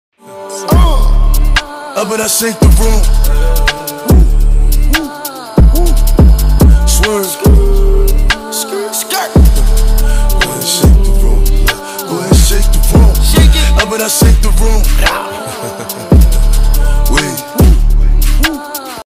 Reversible Burberry & Gucci Mask
Reversible mask so you can be stylish two different ways🤩 All custom made 🤔 we make all kinds of mask